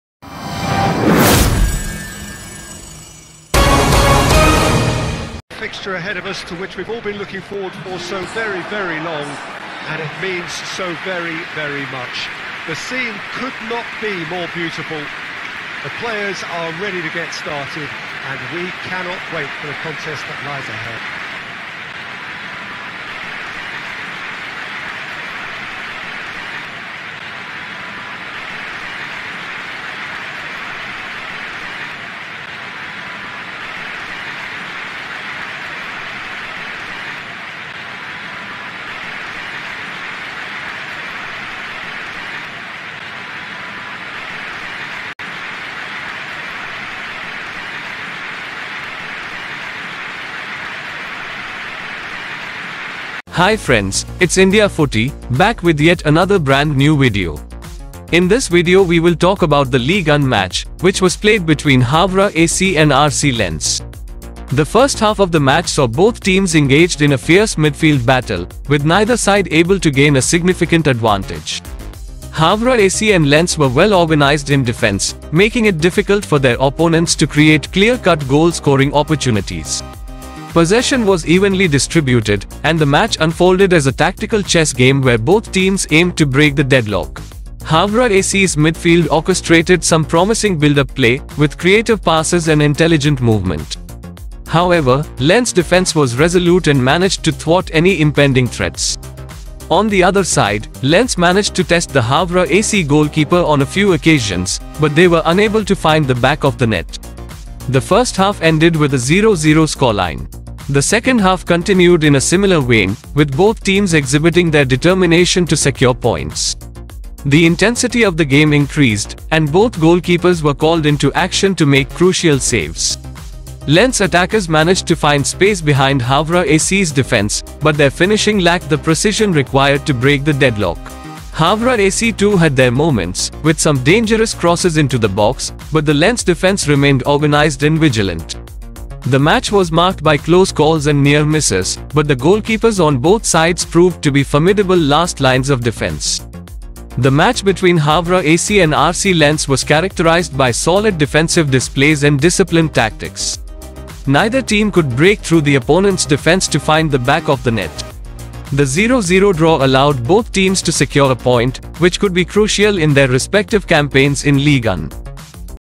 Le Havre - Lens | 0-0 | Le résumé | Ligue 1 2023/24
Le match entre le Havre AC et le RC Lens a été caractérisé par de solides performances défensives et une tactique disciplinée. Aucune des deux équipes n'a réussi à percer la défense adverse pour trouver le fond des filets. Le match nul 0-0 a permis aux deux équipes de prendre un point, ce qui pourrait s'avérer crucial dans leurs campagnes respectives en Ligue 1.